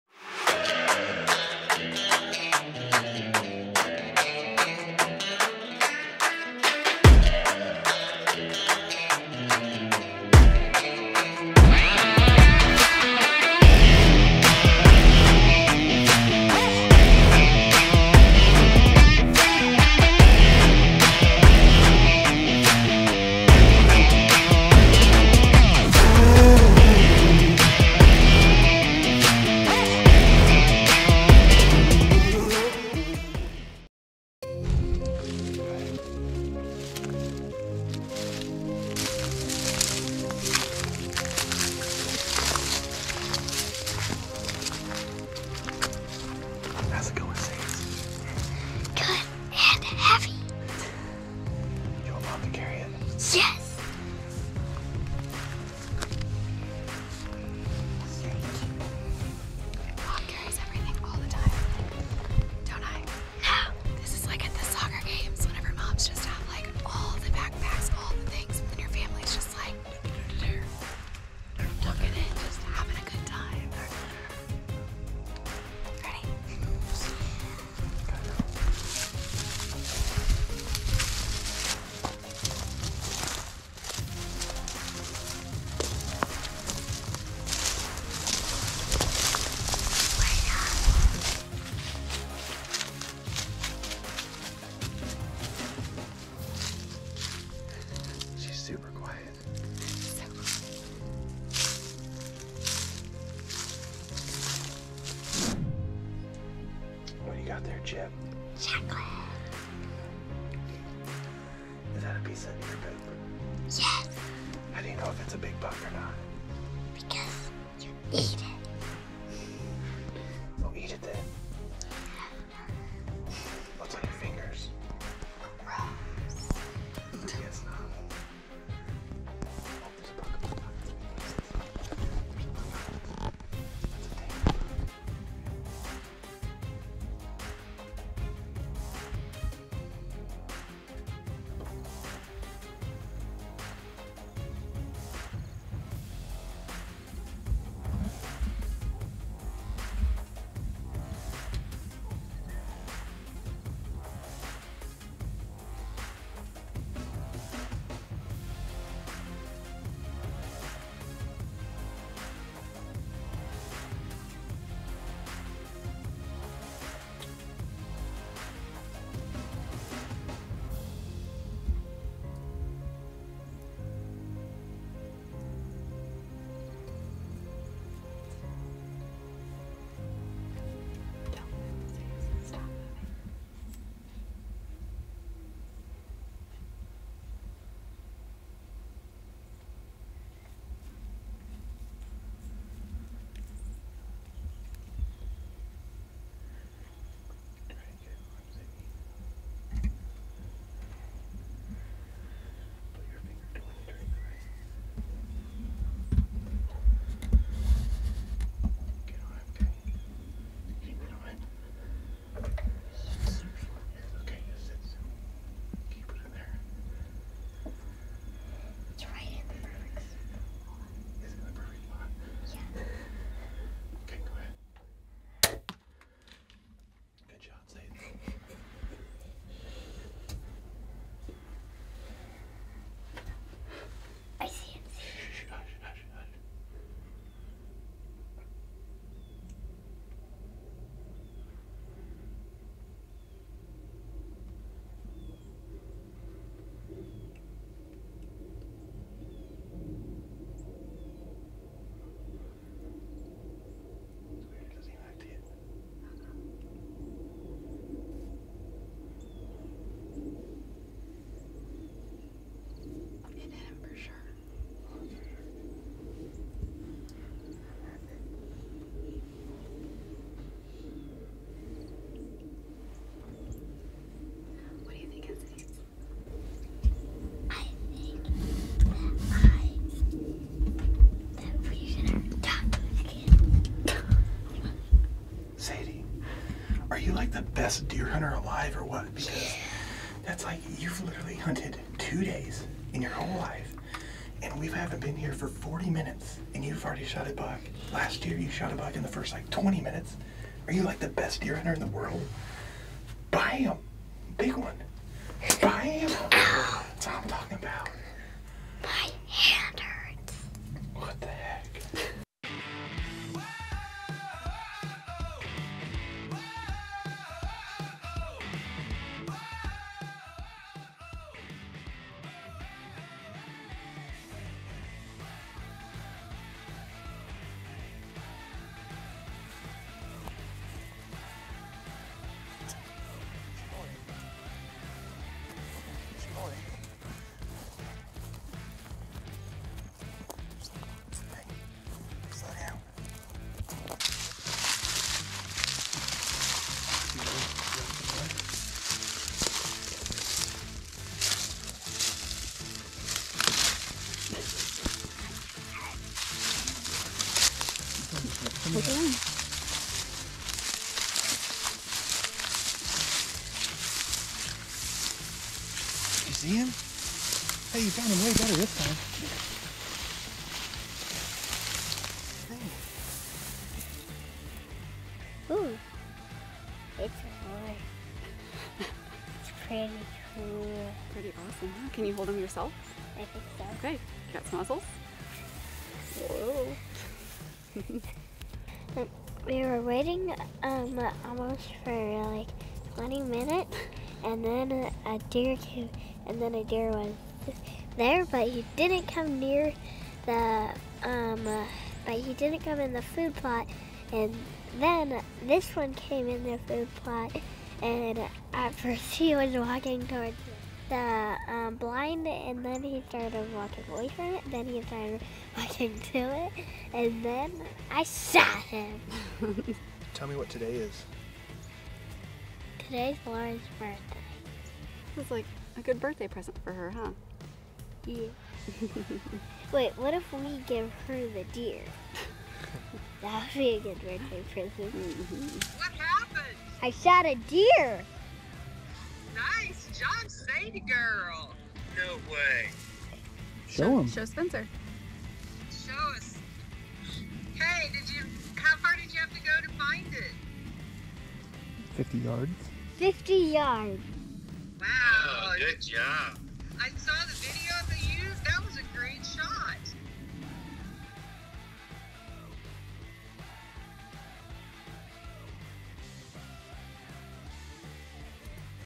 6 year old girls scores second buck in Kansas!
6 year old Sadie goes after her second ever buck in Kansas! After scoring a great buck last year with her crossbow she looks to add another trophy to the wall!